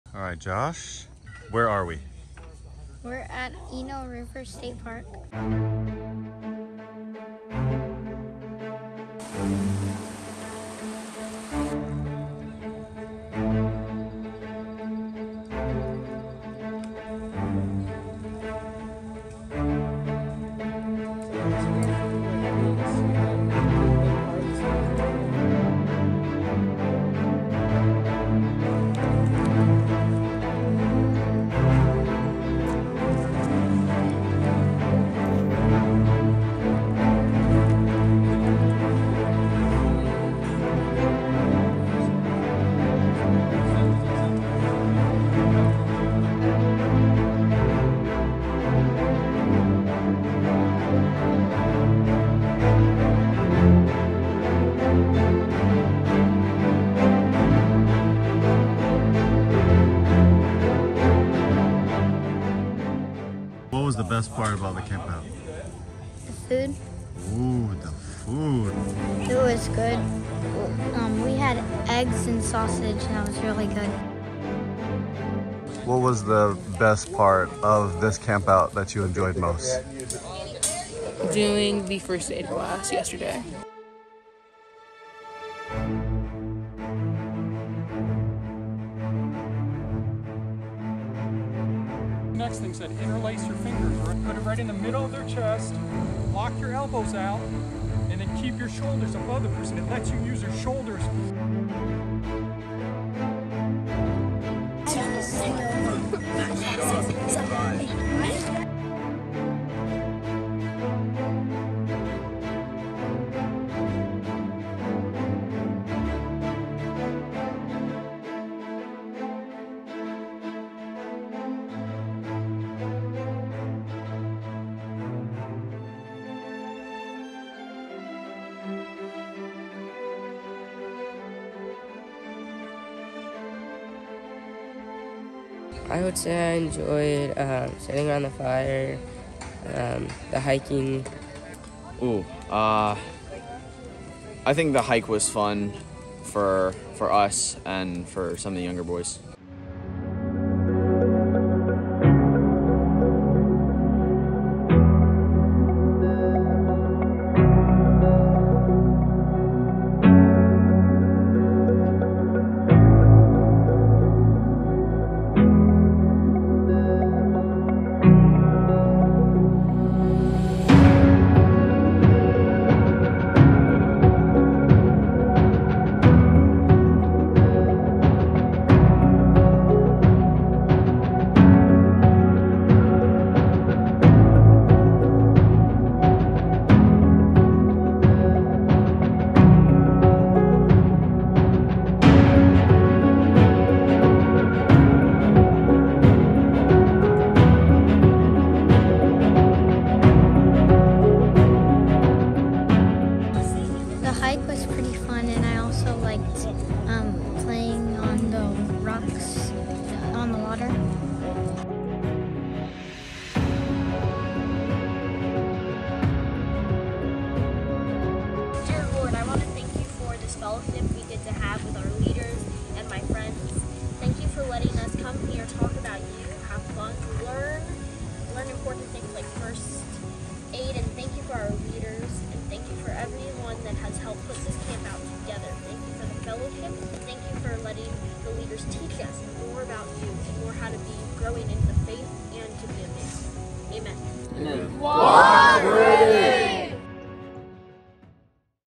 ENO River Campout - Trail Life Boys - Troop NC 4316
In this video a troop of boys who are apart of a Christ centered boys group called Trail Life (a Christ-centered, boy-focused outdoor adventure program and ministry). went camping at ENO River. It consisted of the younger boys to older teen boys (Navigators & Adventures).

What Trail Life is about:
Young Trailmen engage in hands-on experiences that develop character, deepen faith, and hone practical skills in the seven program branches (Outdoor Skills, Life Skills, Heritage, Hobbies, Values, Science & Technology, and Sports & Fitness).